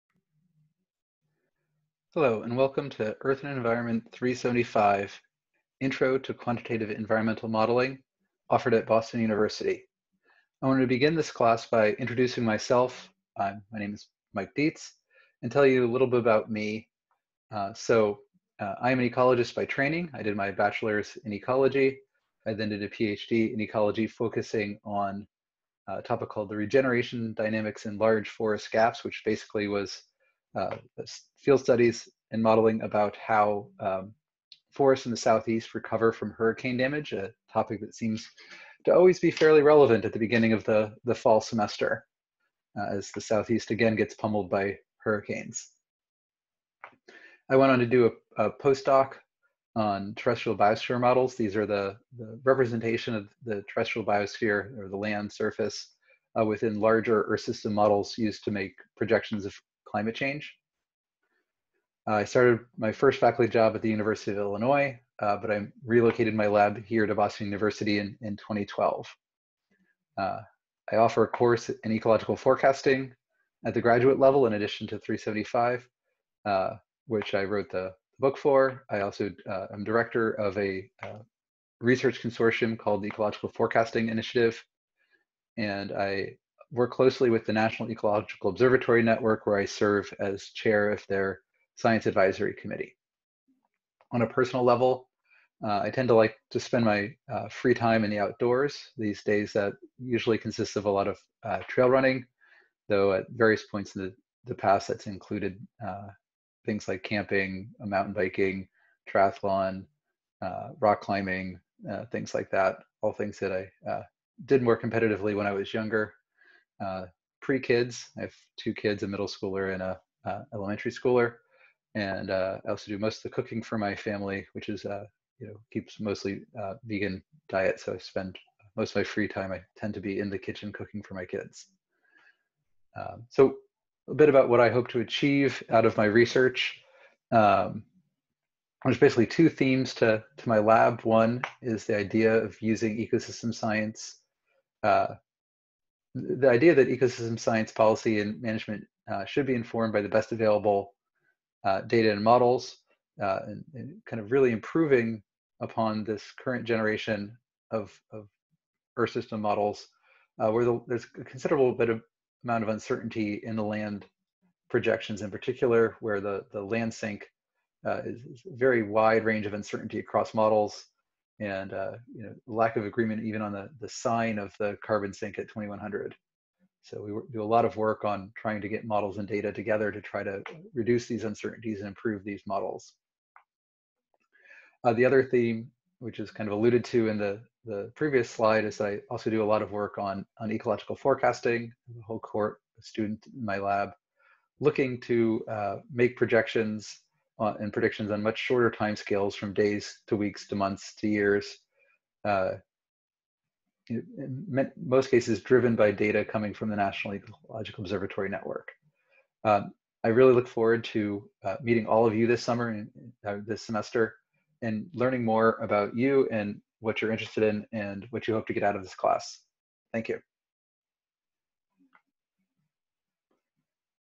EE375 Lecture 1a - Welcome
EE 375 Intro Environmental Modeling: Lecture 1a - Welcome and introduction from the instructor, Michael Dietze. Fall 2020, Boston Uniersity.